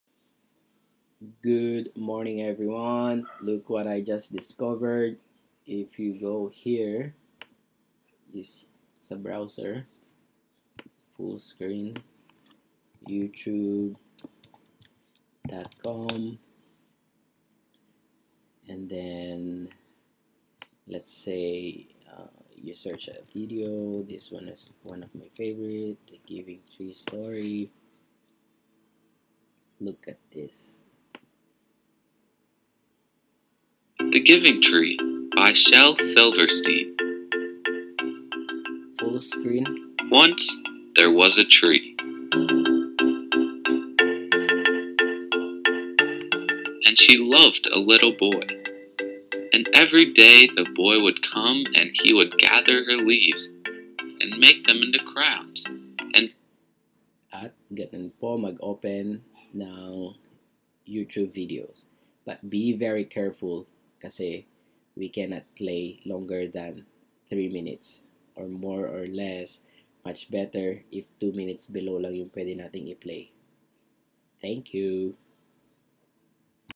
How to play youtube videos using classin.
Click "Teaching tools"
Then "Browser" - globe icon.

Remember not to play for more than 3 minutes of videos.